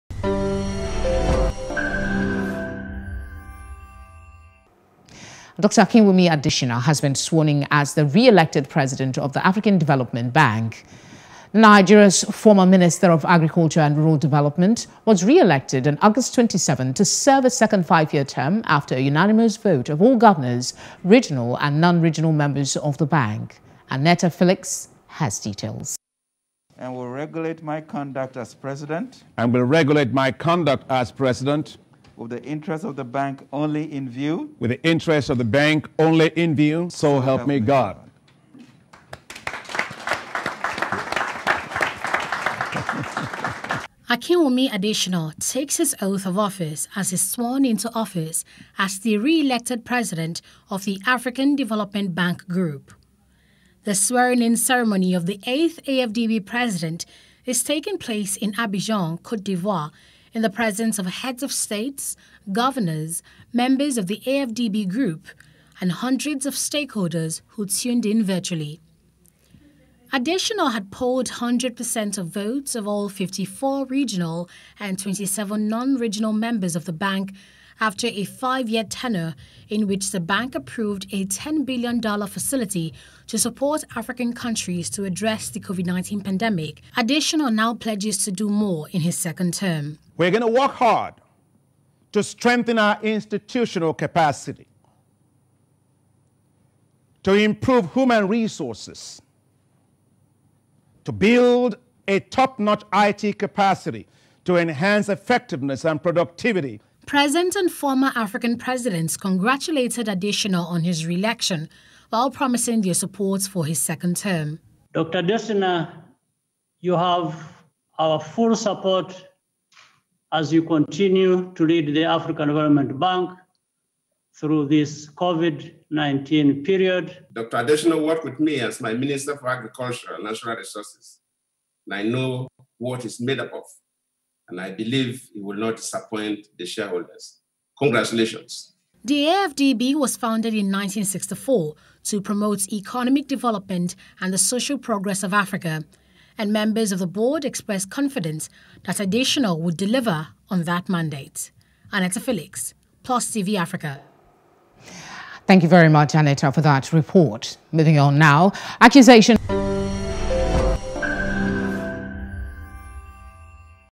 Akinwunmi Adesina Sworn-in for a Second Term as AFDB President
Akinwumi Adesina has been sworn-in as the re-elected President of the African Development Bank.